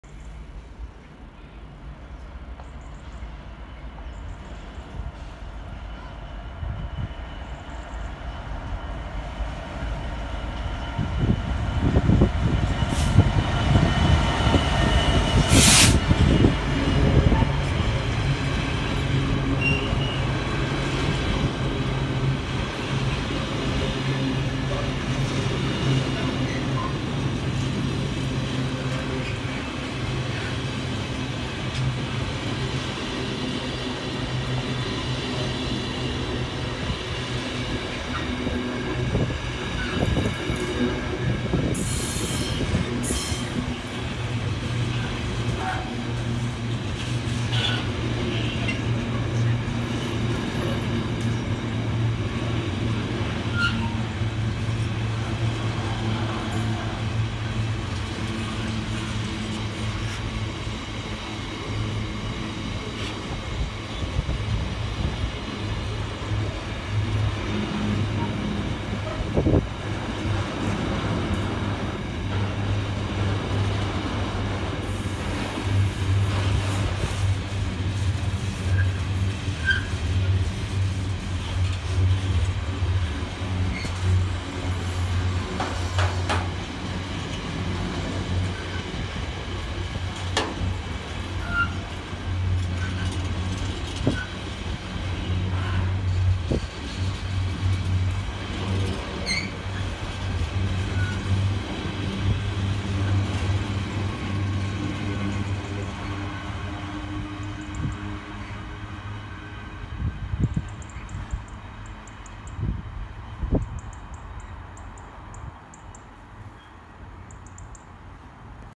66782 Charity Railtours towing 36 mixed containers Tinsley to Felixtowe 13:36 Lea Road 12/2/2024
66782 Charity Railtours towing 36 mixed containers 4L65 Tinsley yard to Felixtowe north 13:36 running 30 minutes early Lea Road Gainsborough 12/2/2024
Containers include Maersk, Qube, Triton, P&O Nedlloyd and Hamburg sud